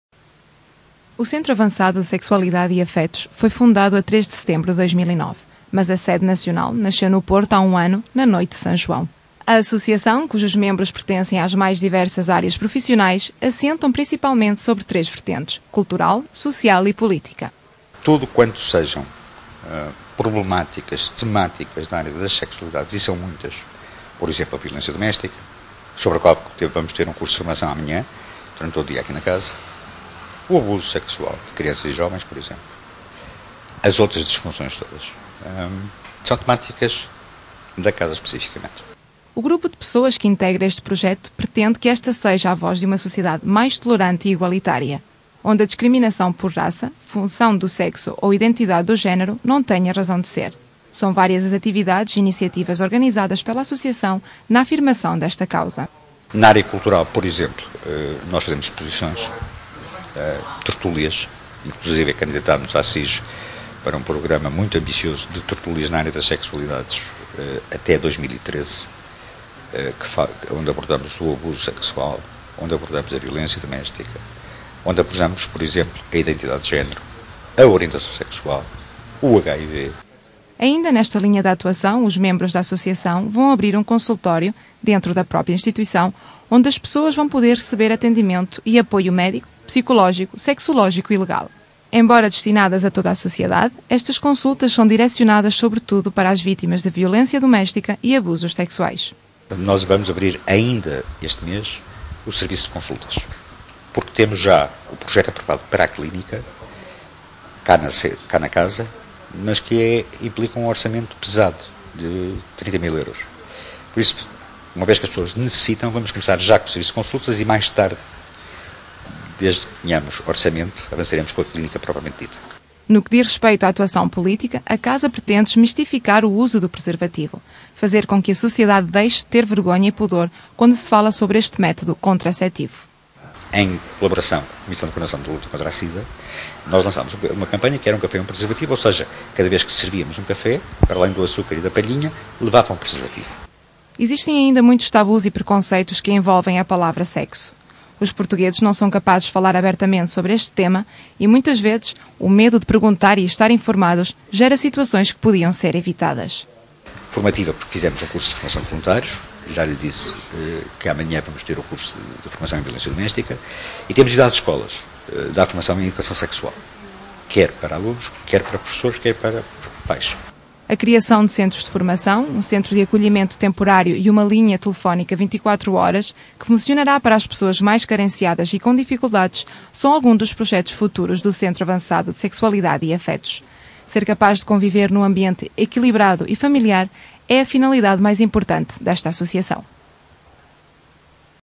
Centro Avançado de Sexualidades e Afectos
No Porto existe uma Associação sem fins lucrativos  que tem por objectivo denunciar todas as formas de descriminação e lutar pela Universalidade dos Direitos à Felicidade. Chama-se Centro Avançado de Sexualidades e Afectos e está na Invicta há um ano.